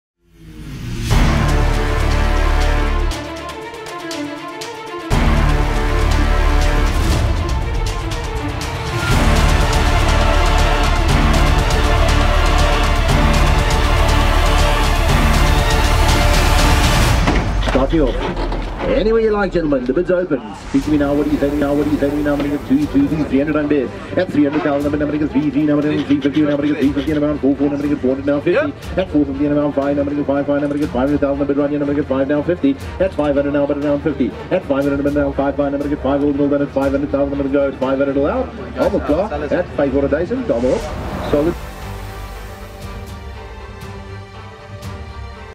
Truck, Trailer & Construction Auction 28 feb
-28 FEB-
Truck, Trailer & Construction Auction-
De-Fleet Auction!

When: Thursday 28 FEBRUARY 2019 @ 10:30
Viewing: Wednesday 27 FEBRUARY 2019 @ 08:00 to 17:00

Venue: Plot 292 Rietfontein Muldersdrift / Gauteng

Devon Le Roux - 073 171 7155